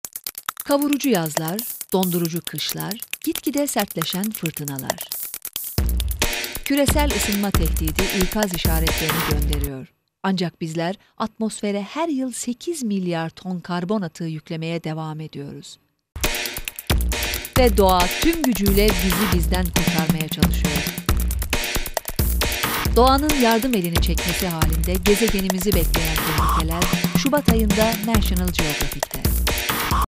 Carbon Emissions - National Geographic Magazine Turkey TV Commercial - February 2004
Voice over in Turkish, National Geographic Turkiye TV Commercial, 30 sec. Video editing and music mixing done by myself. (Baha Ata aka Max Crandale) All other rights owned by respected owners.